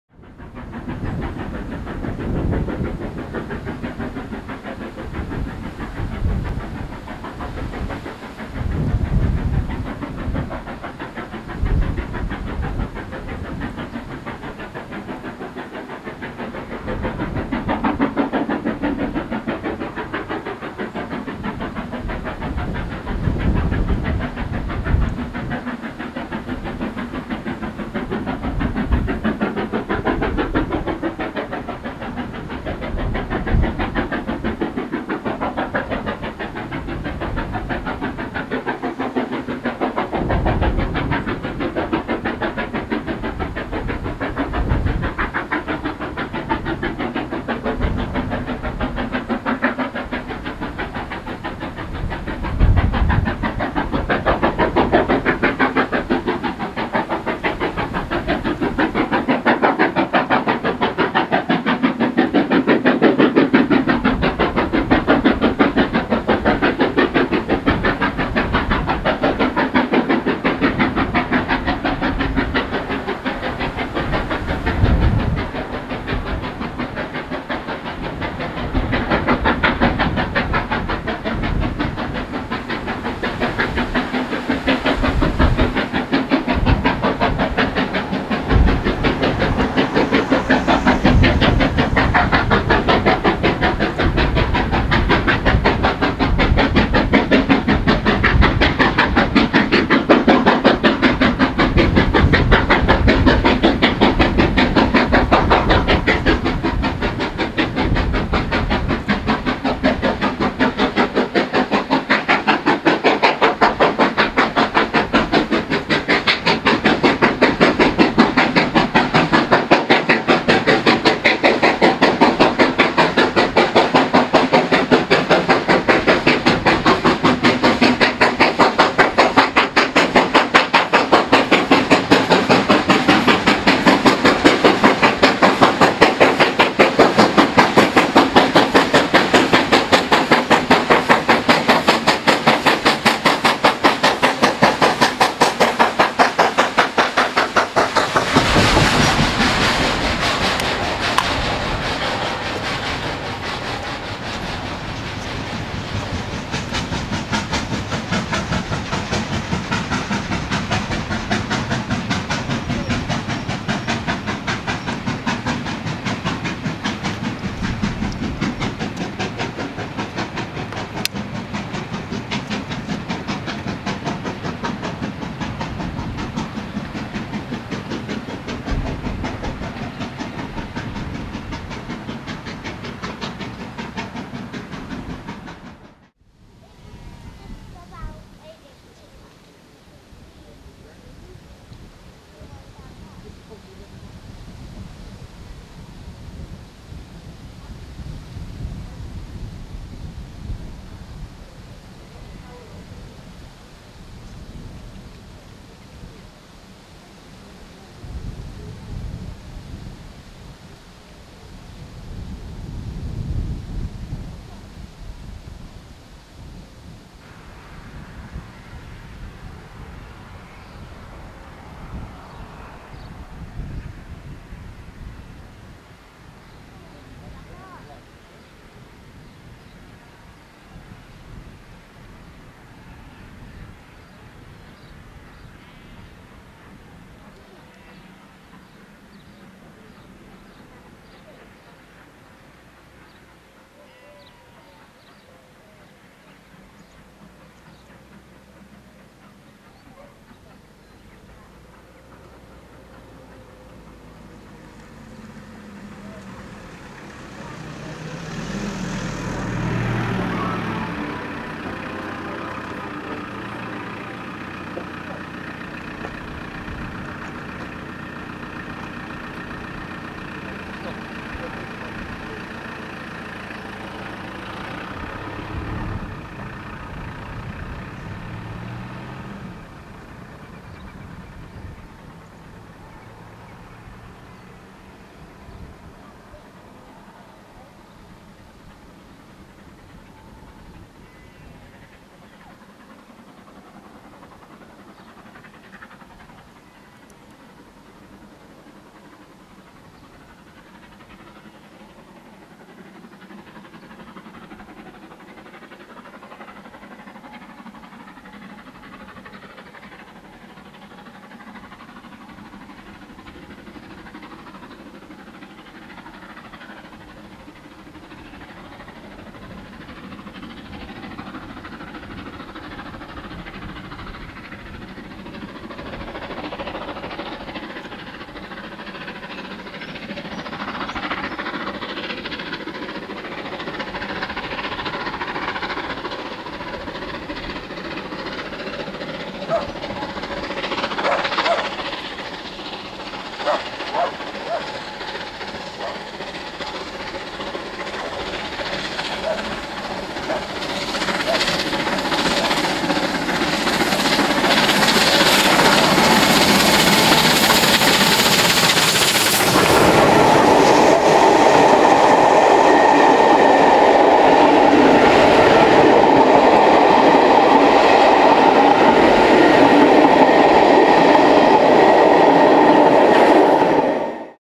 The Cathedrals Express to Weymouth - 9th July 2011
SR Westcountry 34067 Tangmere hauled the train from London to Weymouth and back from Yeovil Junction.  LMS 8F 48151 hauled the train from Weymouth to Yeovil Junction.
The first clip is 48151 climbing to Bincombe Tunnel after leaving Weymouth.  The second clip is 34067 Tangmere, just after leaving Yeovil Junction, on the approach to Bradford Abbas.